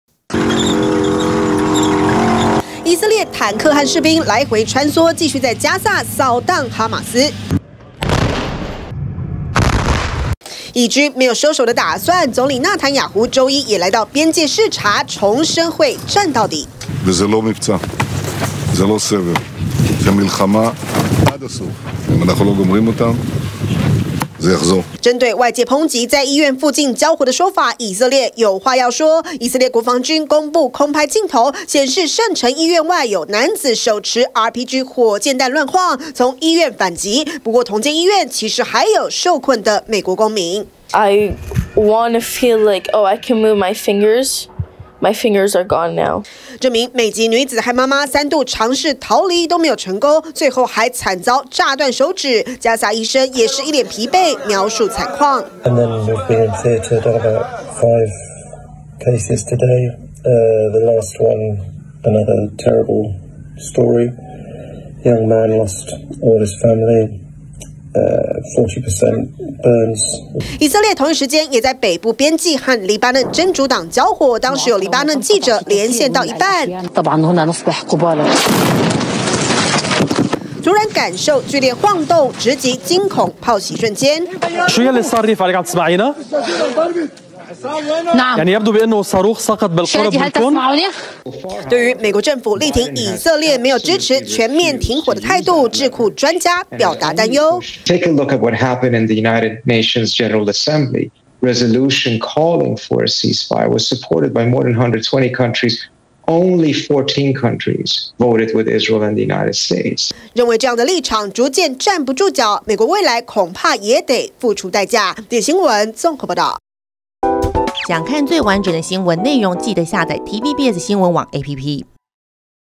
以色列總理稱戰到底 再抓包哈瑪斯躲醫院｜TVBS新聞
針對加薩醫院周遭，持續交火的現象，以色列面臨壓力，也公布一段，在「聖城醫院」的空拍，影片拍到有哈瑪斯成員，手持武器在醫院門口穿梭。然而戰火之下，受害的無辜平民實在太多，還有美籍母女三度逃難不成，被炸斷手指。另外以黎邊境也持續交火，有記者連線到一半，驚險直擊砲襲瞬間。